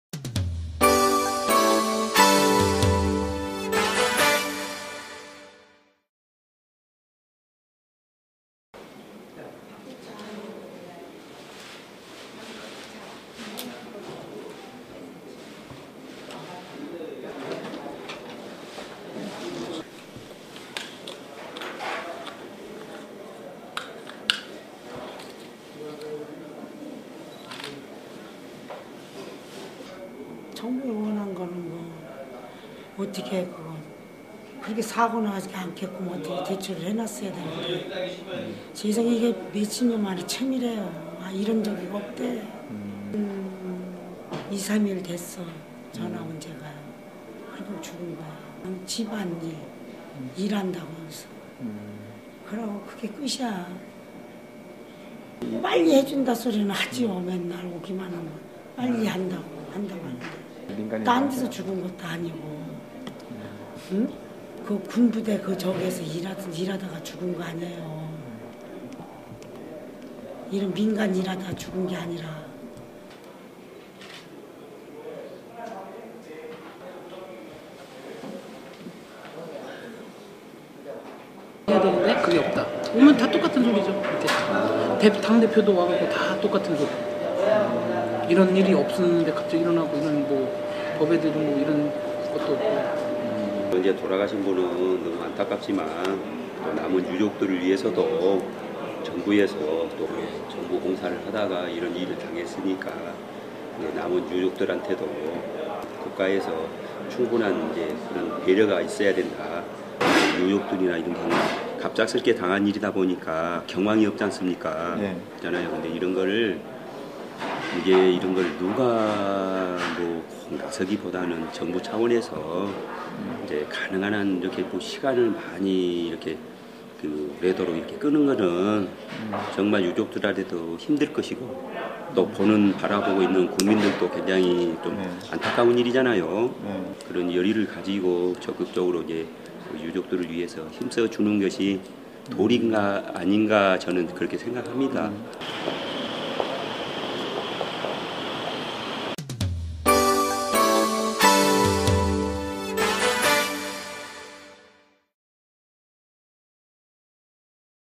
[천지TV]연평도 민간인 희생자 '고 김치백, 고 배복철 씨' 빈소 현장
연평도 민간인 희생자 '고 김치백, 고 배복철 씨' 빈소 현장
인천 길병원 장례식장

[인터뷰: 배복순(64) | 고 배복철 씨 누나]
정부에 원하는 것은 그렇게 사고가 나지 않게 대처를 해놨어야 하는데...
세상에 이게 몇 십 년 만에 처음이래요. 이런 적이 없대...

2, 3일 됐어. 전화 온 지가. 그리고 죽은 거야. (전화 내용은)집안 일, 일 한다면서. 그리고 그게 끝이야.

(정부에서는) 빨리 해준다고는 하지요. 오기만 하면...
딴 데서 죽은 것도 아니고 군부대에서 일 하다가 죽은 것 아녜요. 민가에서 일 하다가 죽은 게 아니라.

[인터뷰: 배진엽(25) | 고 배복철 씨 조카]
오면 다 똑같은 소리죠. 당 대표도 와서 다 똑같은 소리고 "이런 일이 없어야 하는데 생겼고, 법에도 이런 사례가 없다"고...

[인터뷰: 박정훈(54) | 고 김치백 씨 지인]
돌아가신 분은 안타깝지만 남은 유족들을 위해서도 정부에서...
정부공사를 하다가 이런 일을 당했으니까 남은 유족들에게 국가에서 충분한 배려가 있어야 한다.

유족들은 갑작스럽게 당한 일이다보니까 경황이 없잖습니까 이런 걸 누가 나서기보다는 정부 차원에서...시간을 많이 끄는 것은 유족들도 힘들 것이고 , 보는 국민들도 안타까운 일이잖아요.

열의를 가지고 유족들을 위해서 적극적으로 힘써 주는 것이 도리가 아닌가 생각합니다.